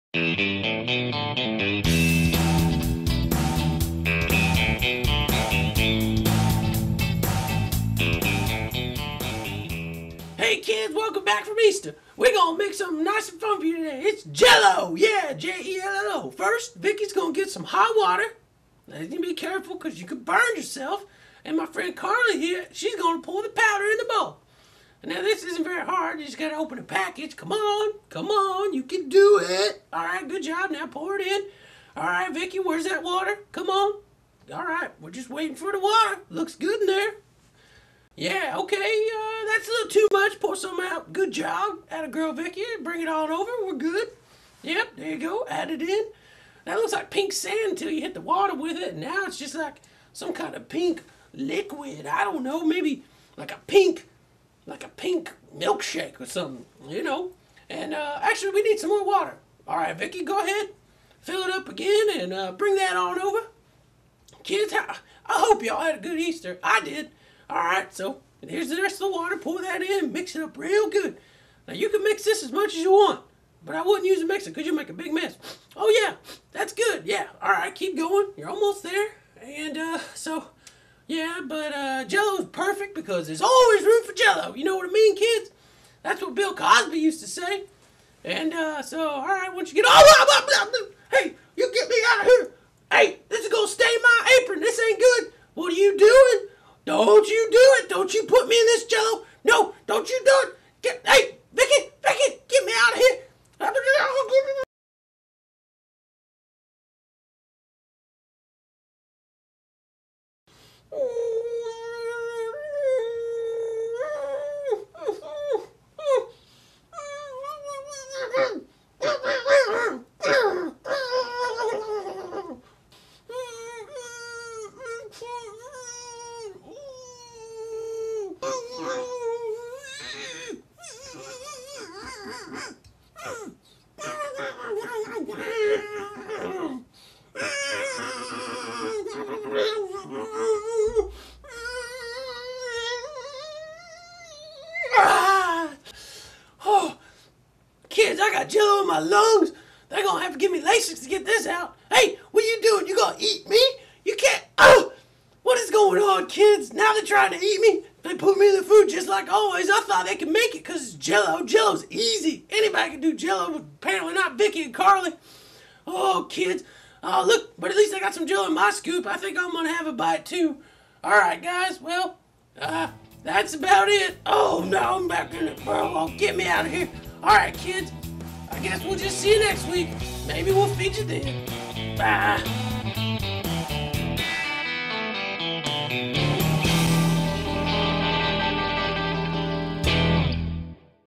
Luncheon' with Linda episode #10
Vicky & Carly messed up pancakes last episode, so Linda tries something even easier...Jell-o.
There's always room for it, and everyone can cook it...right?